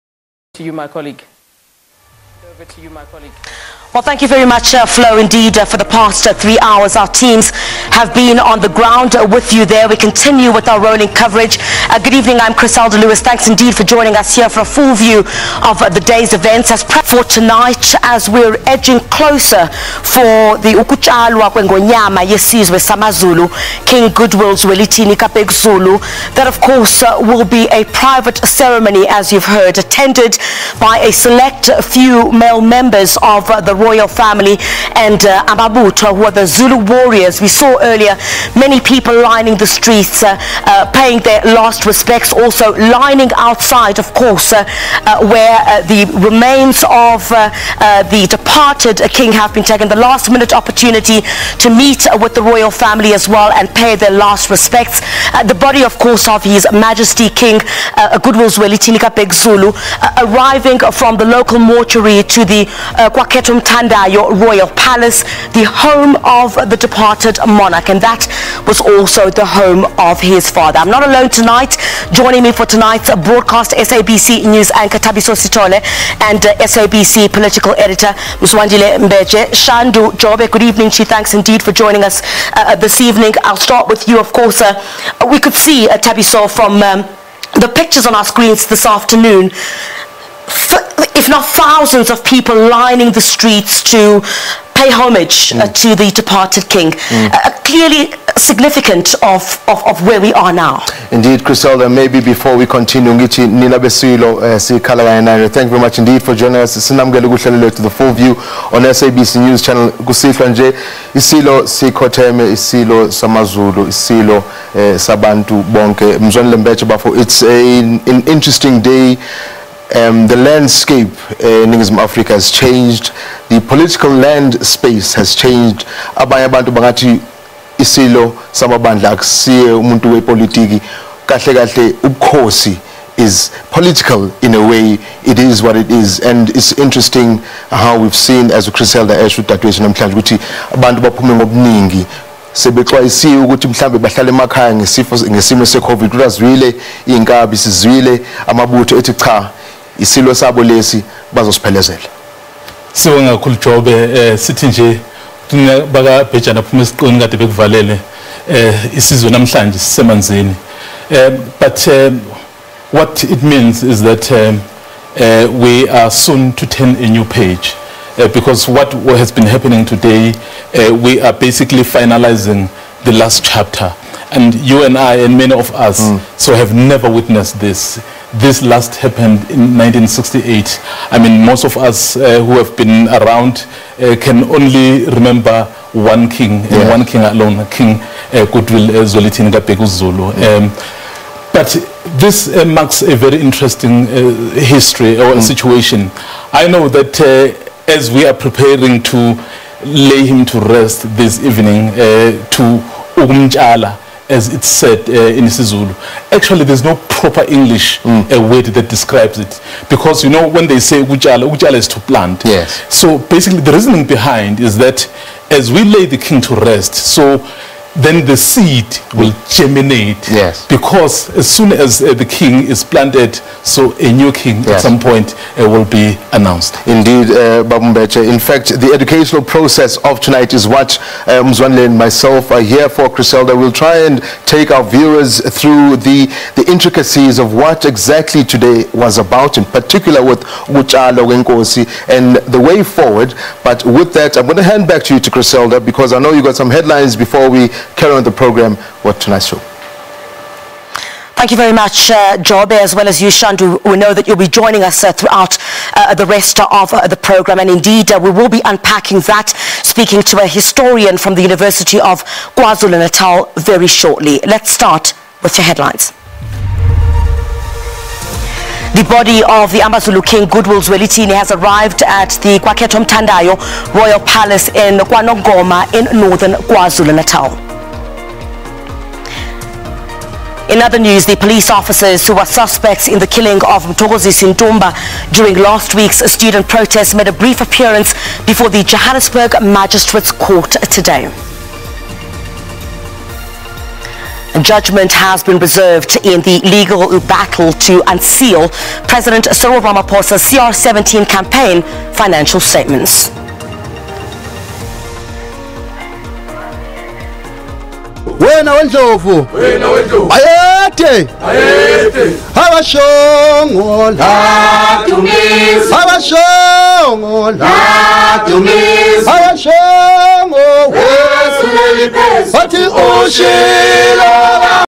SABCNews Headlines @18H00 | 17 March 2021
* The body of amaZulu King Goodwill Zwelithini has arrived at the Kwakhethomthandayo royal palace in Nongoma in northern KwaZulu-Natal.

* The Police officers who are suspects in the killing of  Mthokozisi Ntumba during last week's student protests, made a brief appearance before the Johannesburg Magistrates Court today.

* Judgment has been reserved in the legal battled to unseal President Cyril Ramaphosa's CR17 Campaign financial statements.